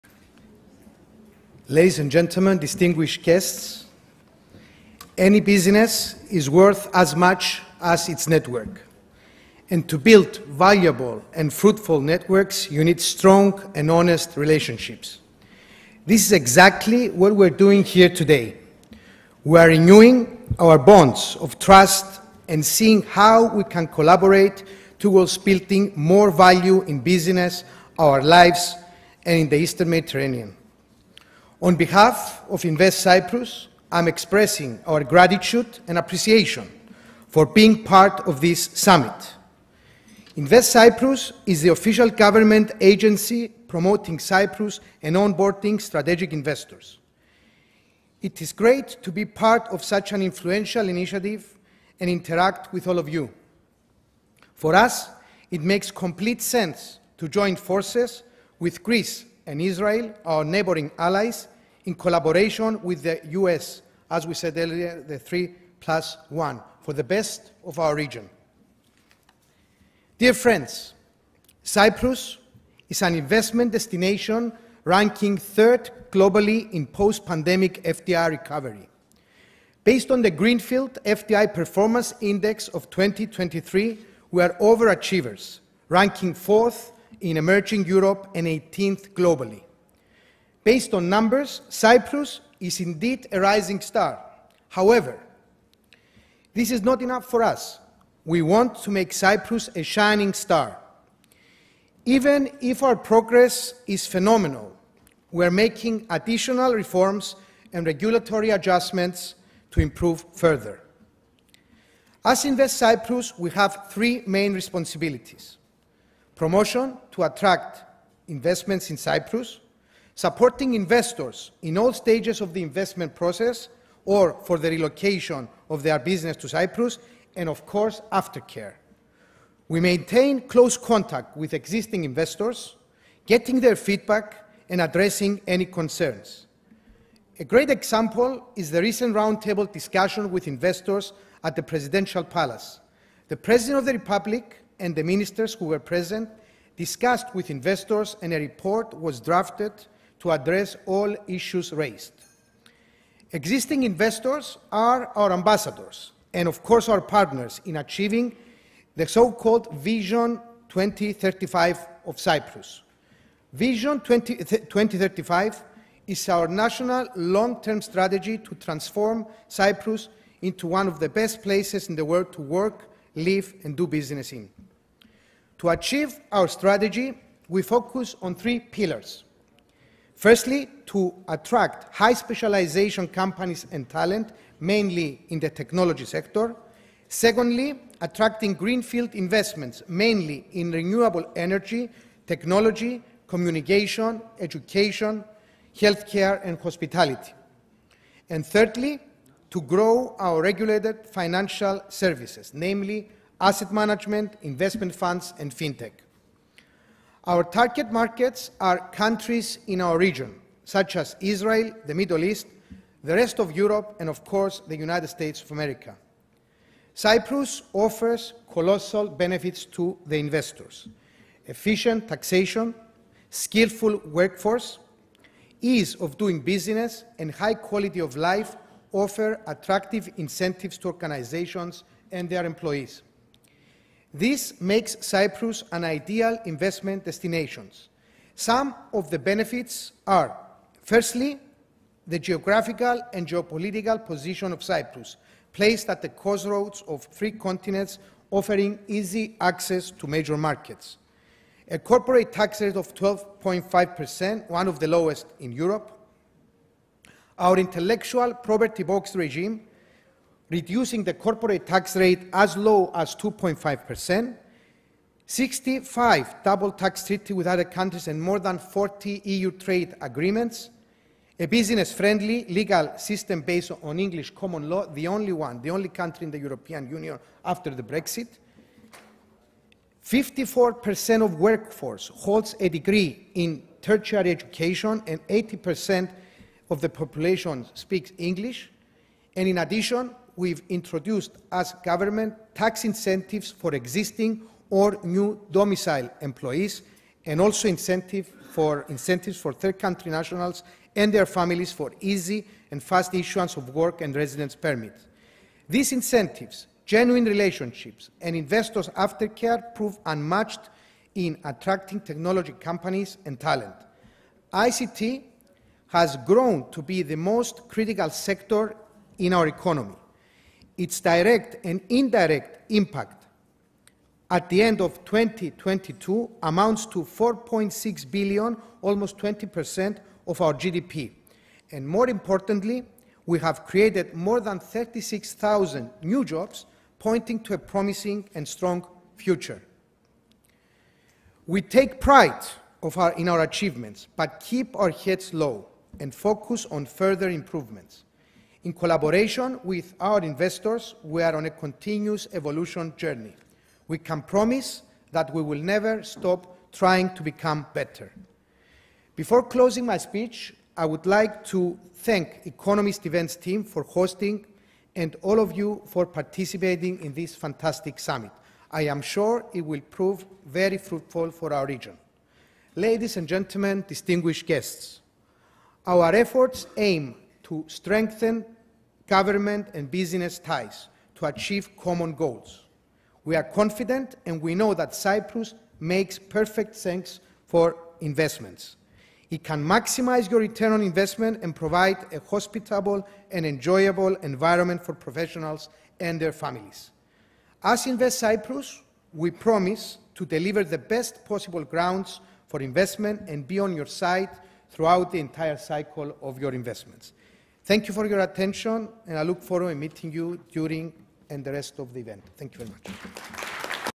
Spyros Vassiliou on THE INVESTMENT CLIMATE IN THE EASTERN MEDITERRANEAN
Spyros Vassiliou, vice-chairman, Invest Cyprus at The Economist Impact's New York – Eastern Mediterranean Business Summit